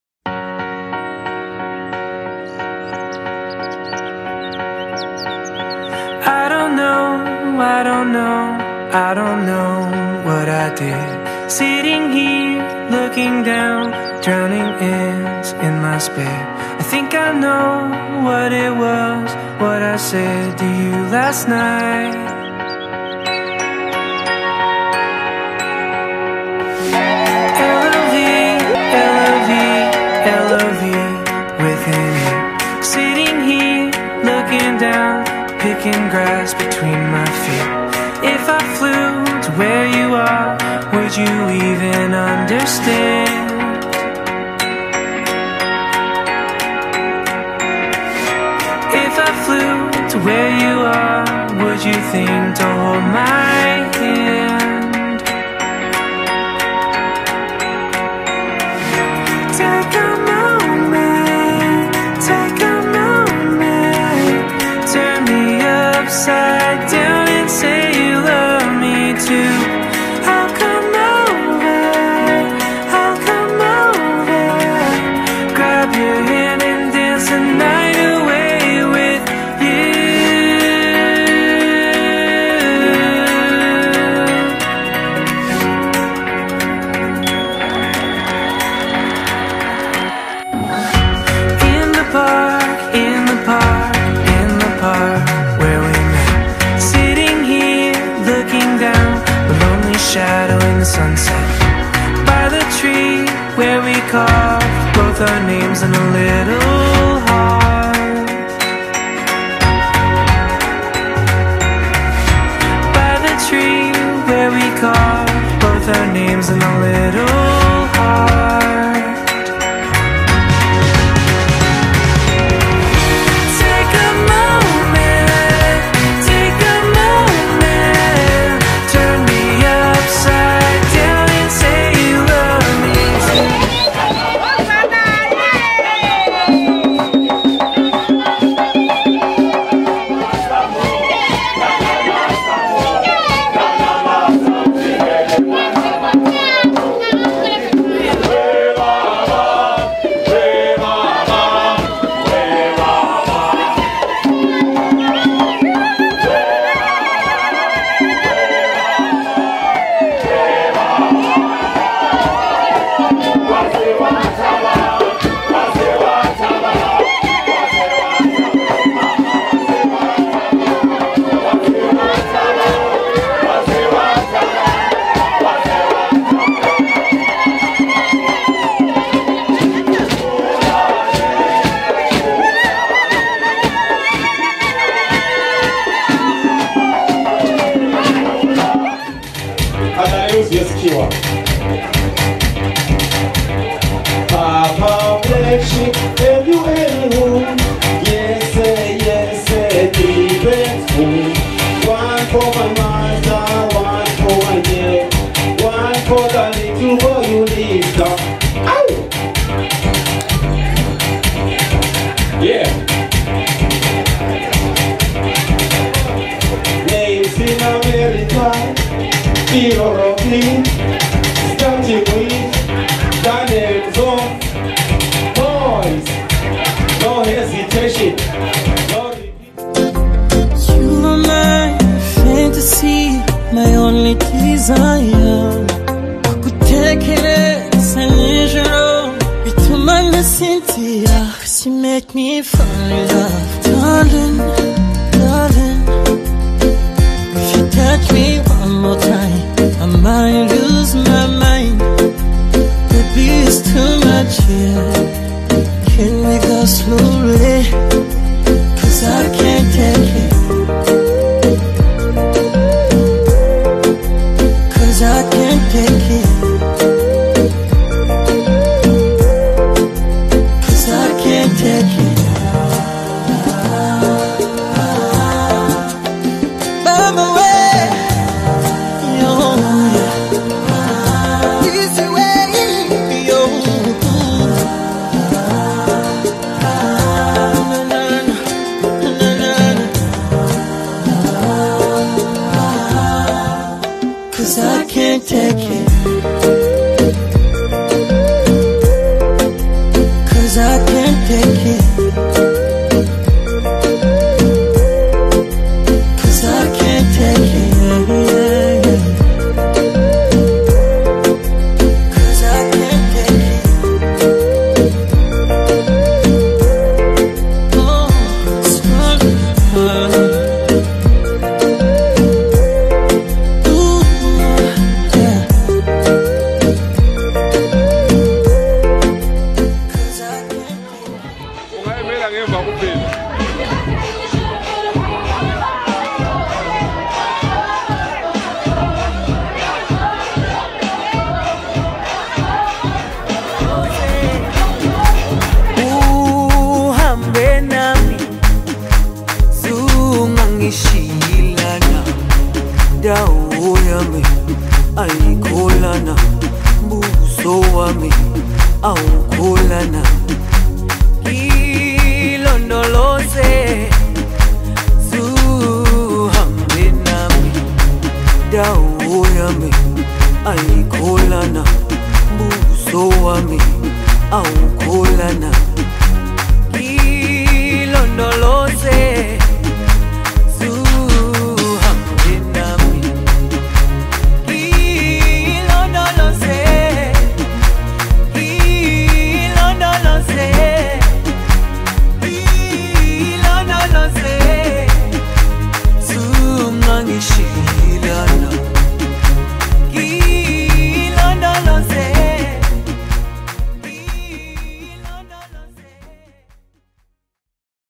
Video Trailer - Becoming the Zulus - Hlanganani and Thenjiwe Wedding
Hlanganani & Thenjiwe wedding at Mystique Lodge Bulawayo Zimbabwe. 28 December 2019.